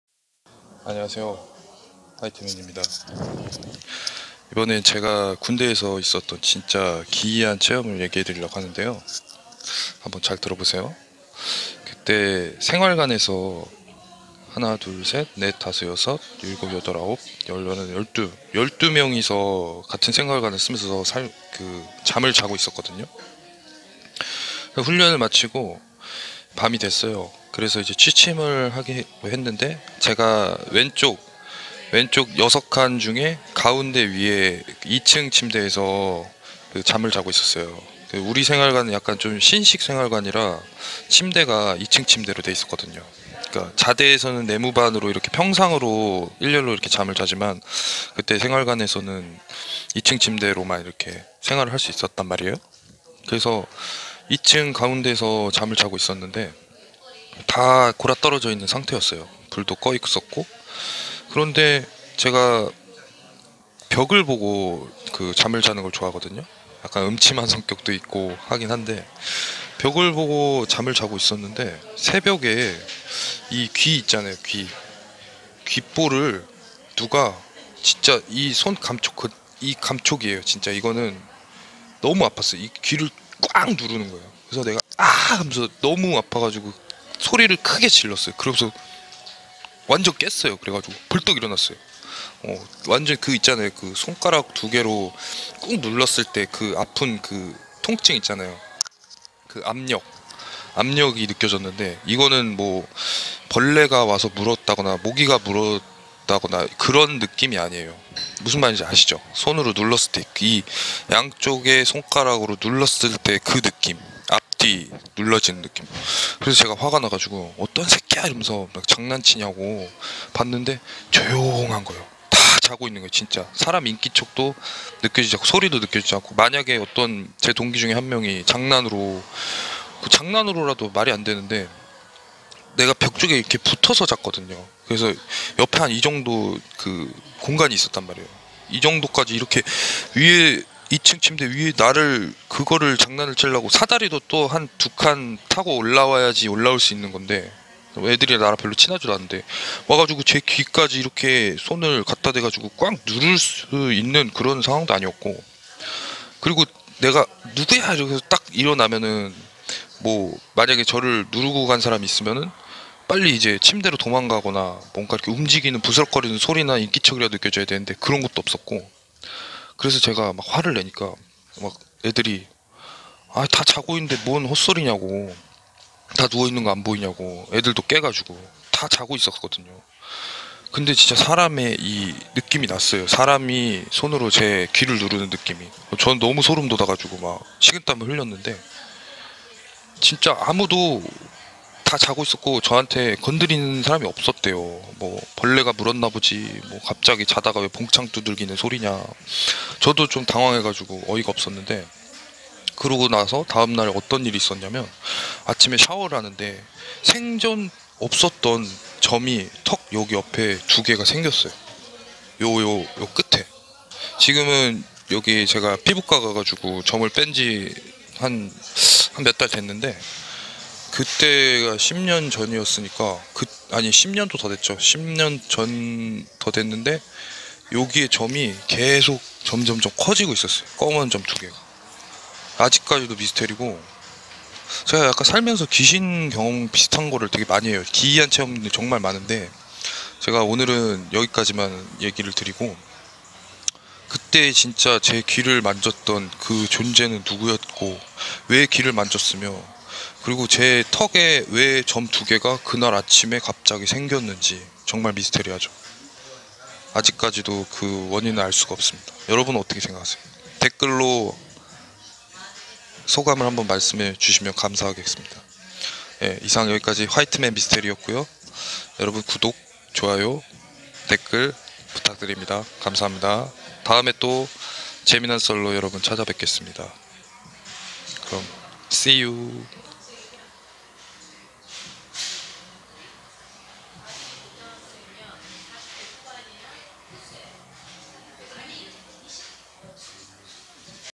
특전사 생활관귀신 오싹!오싹! 소름;; 납량특집 호러,공포 무서운이야기 귀신본썰 (eng sub) The Horror Hand Touch Me;;;
화이트맨 귀신본썰 제2탄!!
특전사 생활관에서 있었던 보이지 않는 "귀신의 손"에 대해서 얘기해 보겠습니다.
제가 잘때 저의 귀를 만진. 그 손은 누구의 손이었을까요? 오싹~!!~